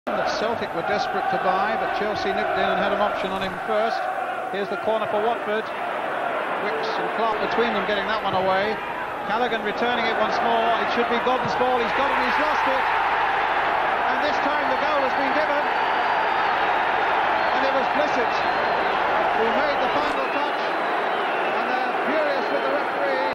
Watford v Chelsea F.A. Cup 4th Round 01-02-1987
Fourth Round tie played on a Sunday. Watford beat non league Maidstone United in the 3rd Round. Chelsea needed a replay to get past Aston Villa.